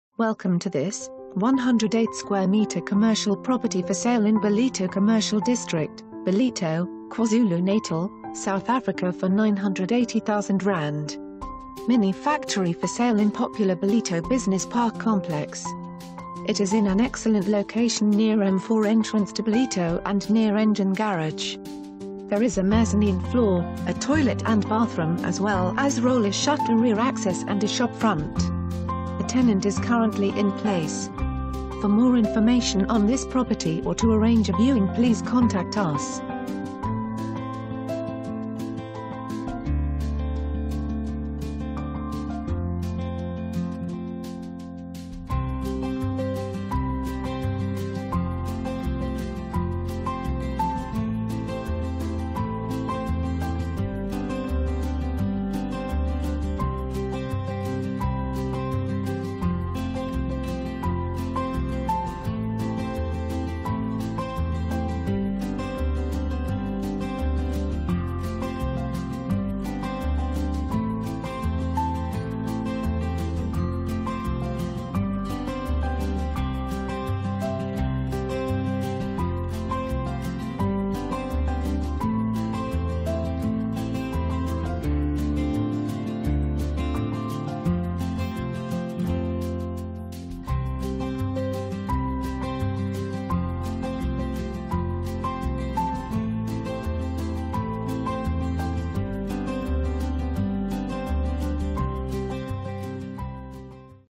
108 m2 Commercial Property For Sale in Ballito Commercial District, Ballito, KwaZulu Natal, South...
Mini-factory for sale in popular Ballito Business Park complex.

It is in an excellent location near M4 entrance to Ballito and near Engen Garage. There is a Mezzanine floor, a toilet and bathroom as well as roller shutter rear access and a shopfront. A tenant is currently in place.